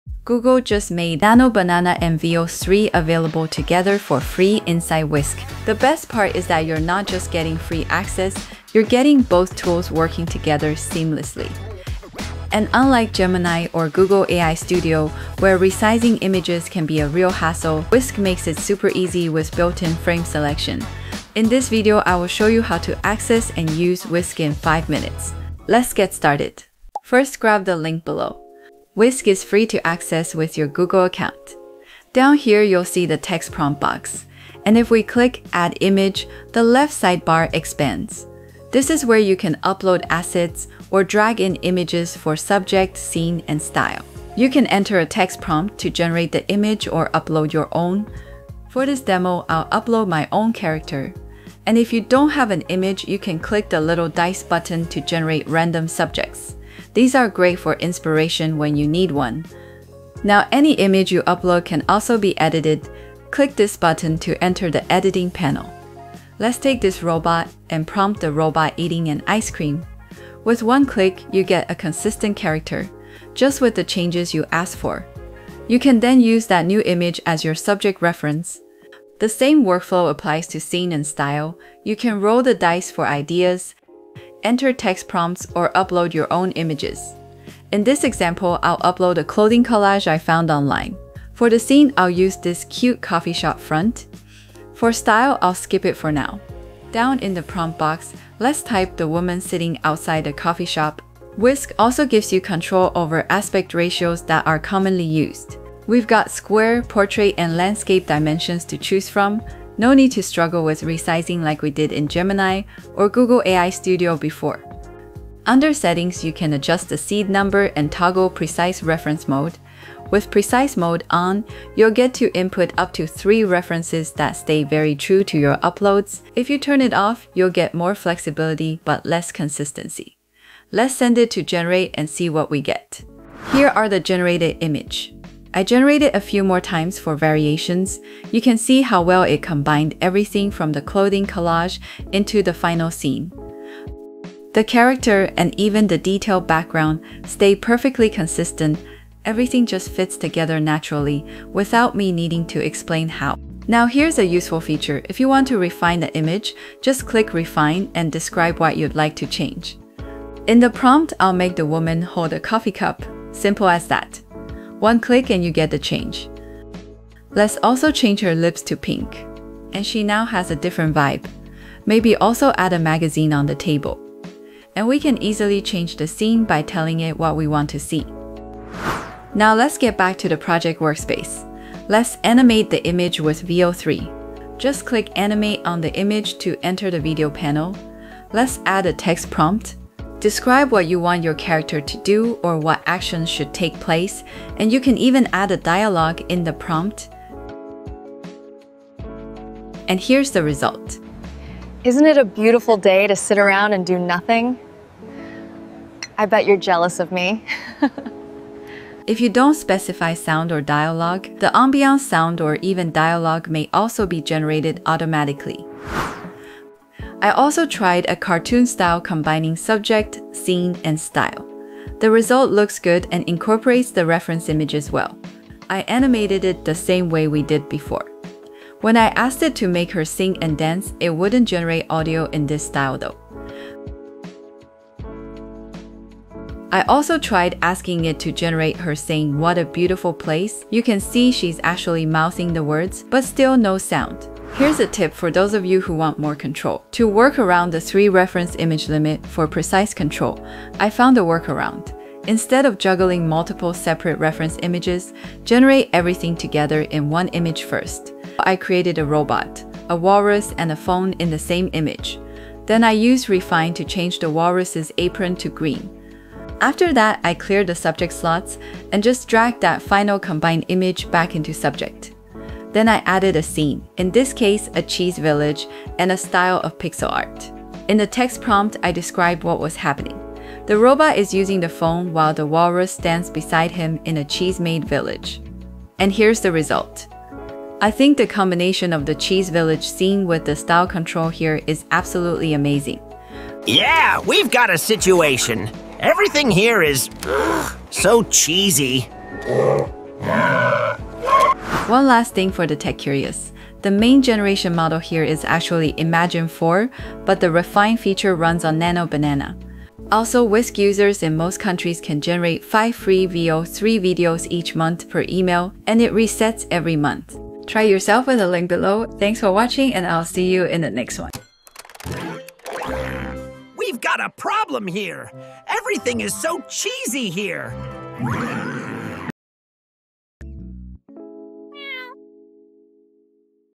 Nano Banana + Veo 3 Now FREE Together in Google Whisk! (5 Minute Tutorial)
*Google just made Nano Banana and Veo 3 available together for free access inside Whisk!*

In this video, I'll show you how to use Google Whisk to generate images and animate them with Veo 3 in under 5 mintues. Unlike Gemini or Google AI Studio where resizing is a hassle, Whisk makes it super easy with built-in aspect ratio controls. Plus, it's so much fun with Whisk!

✨ Want to create your avatar like mine?
I used Heygen for the avatar and the built-in Fish Audio voice cloning inside Heygen.

🔥 Get my AI Toolbox inside my free community.